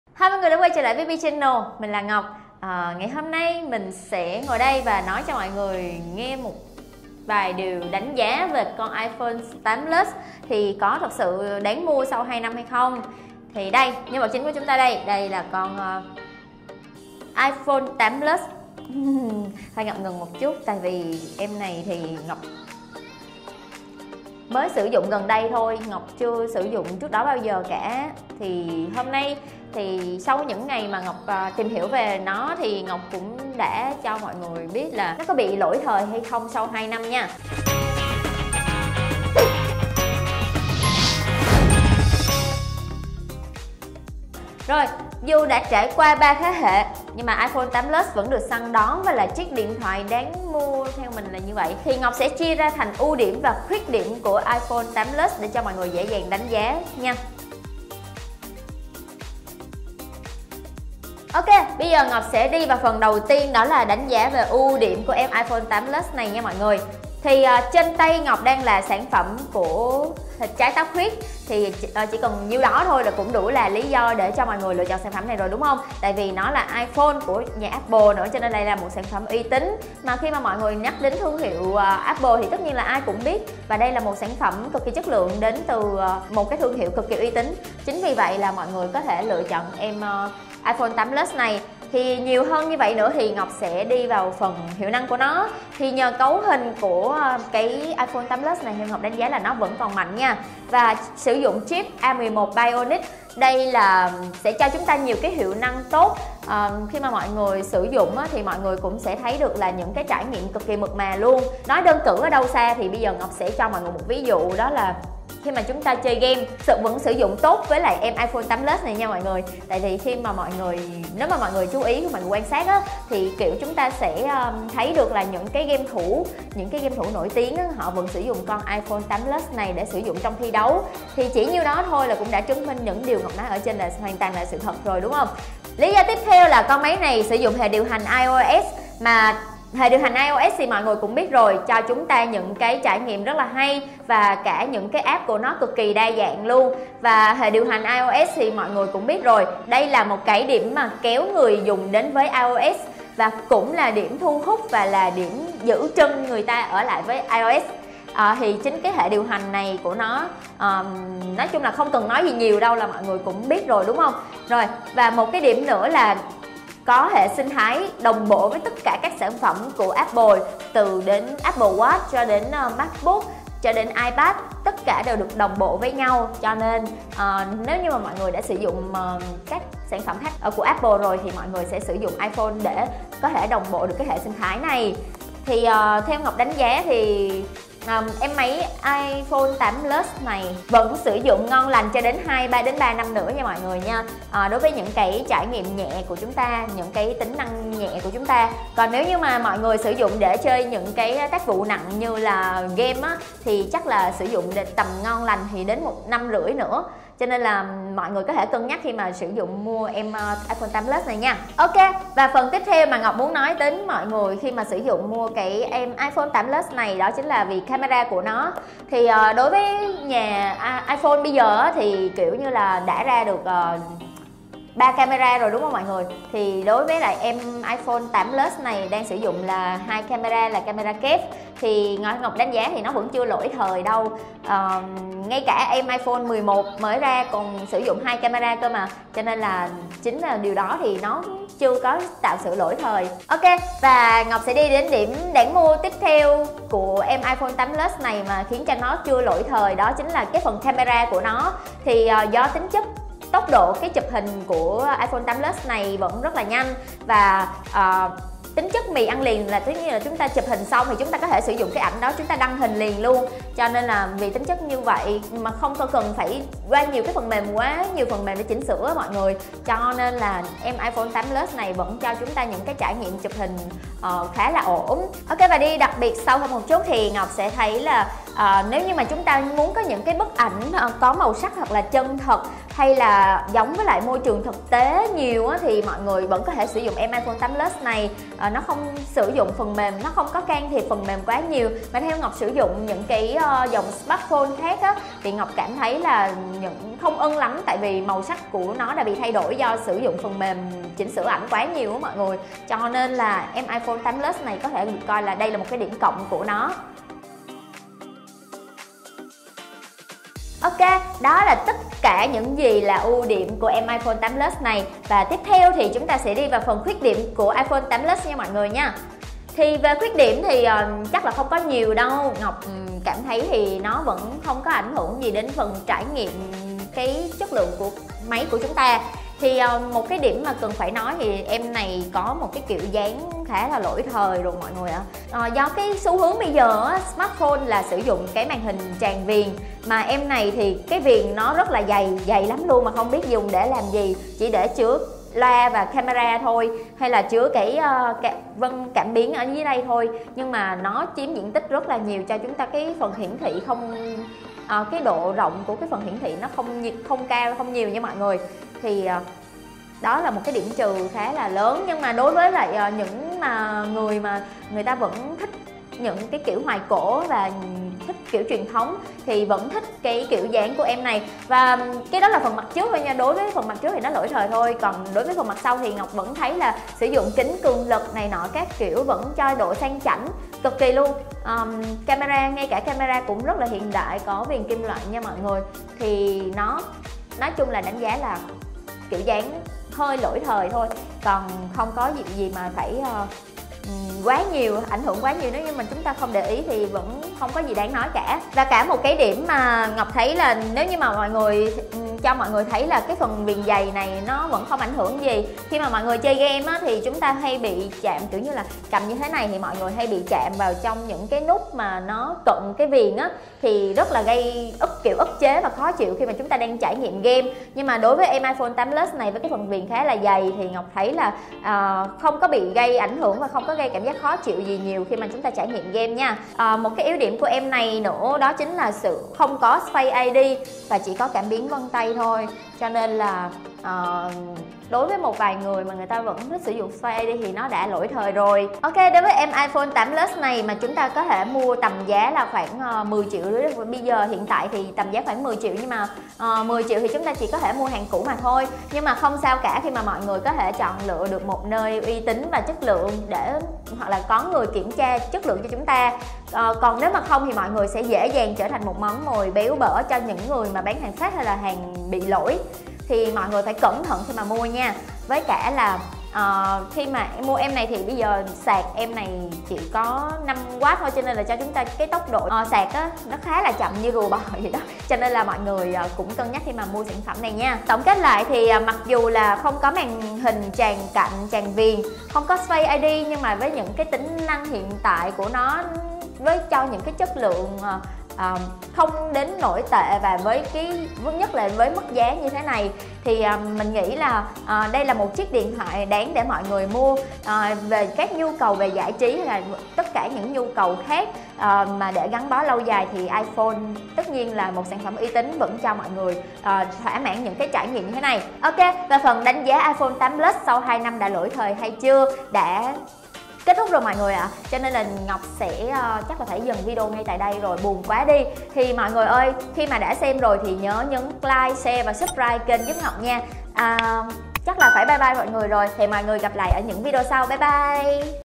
Đánh giá iPhone 8 Plus sau 2 năm ra mắt - Đã lỗi thời ?
Tham khảo mức giá và các chương trình khuyến mại trong video ở link phía bên dưới:

Những sản phẩm của Apple luôn có vòng đời cực kỳ ấn tượng và tất nhiên iPhone 8 Plus - 1 sản phẩm của Apple cũng không phải là ngoại lệ. Sau 2 năm iPhone 8 Plus sẽ phù hợp với đối tượng nào thời điểm hiện tại, chúng ta cùng làm rõ cùng Ngọc nhé!